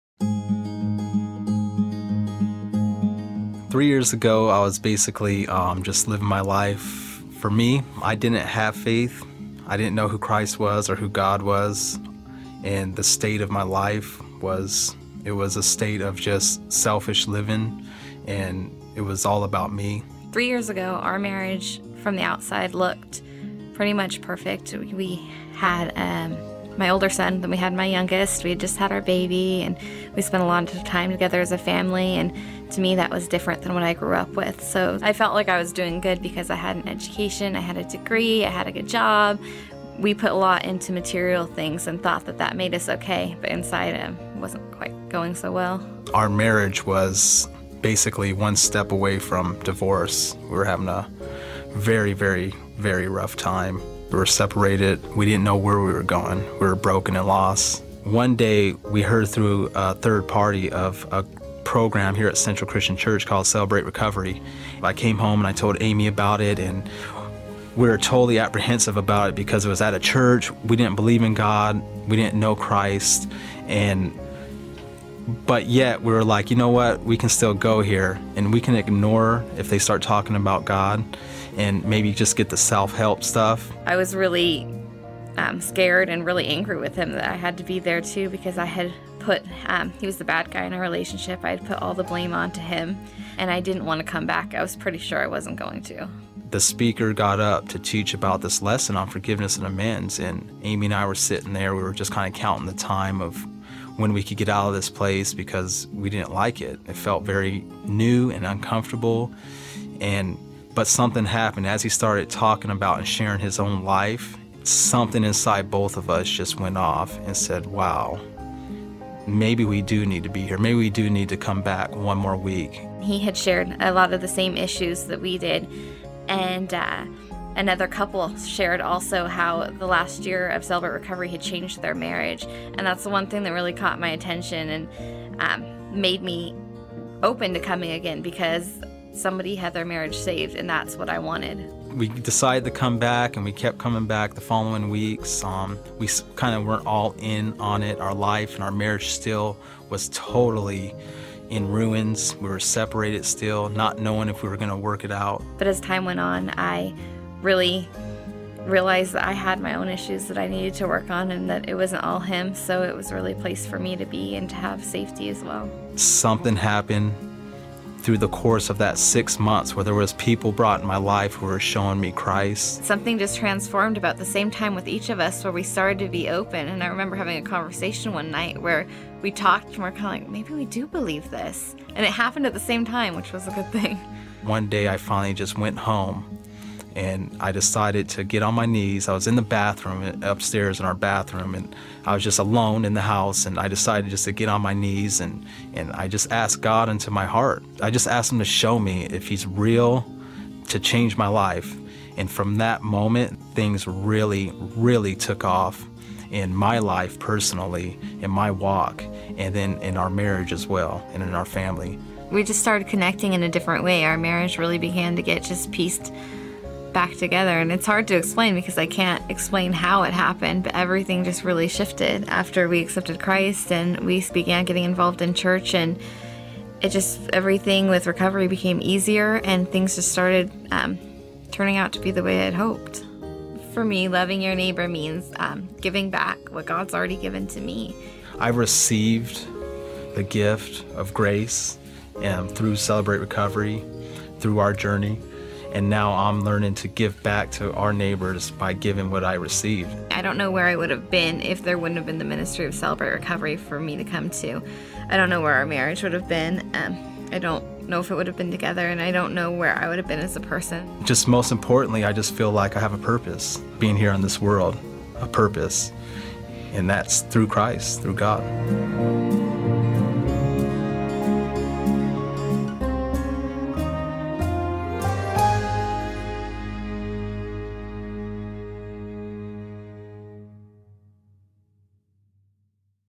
♦2♦ Testimony — Marriage Restored From Divorce {Matt & Amy}
Thanks!